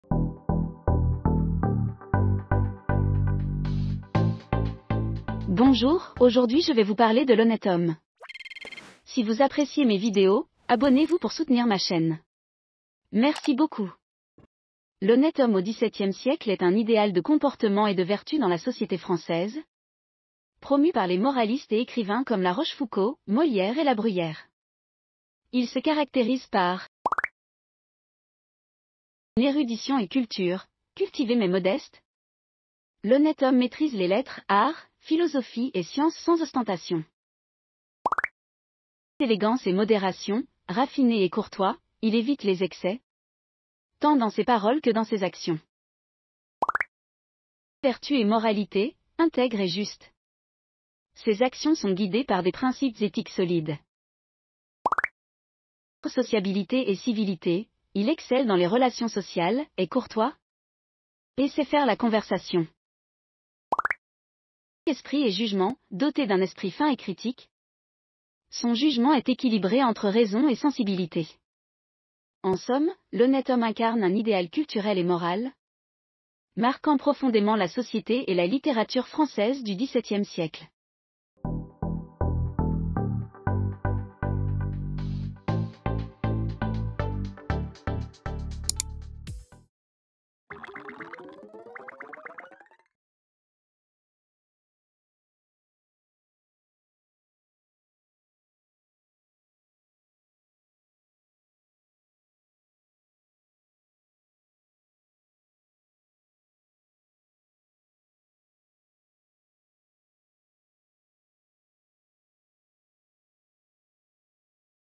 L'honnête homme au 17e siècle
Cette vidéo est une courte présentation de notion littéraire destinée aux étudiants francophones et étudiants de littérature française.

L'honnête homme au 17e siècle représente un idéal de comportement et de vertu dans la haute société.